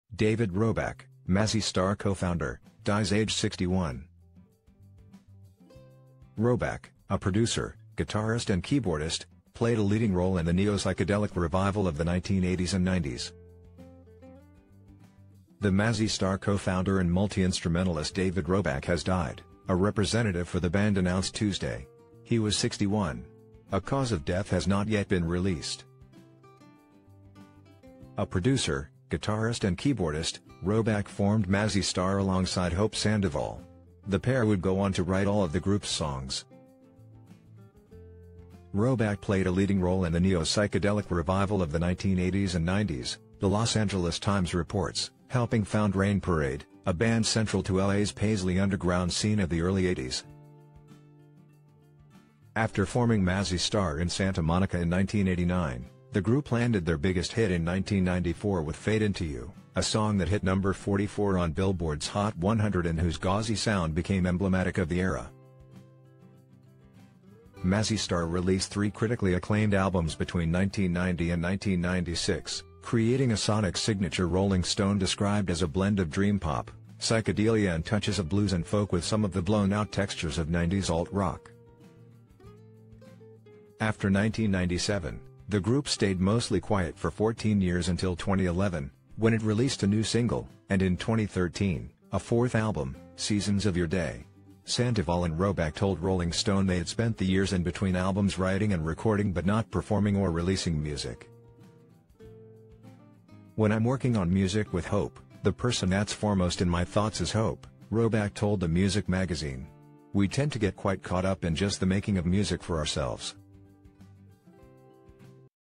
David Roback, Mazzy Star co founder, dies aged 61
David Roback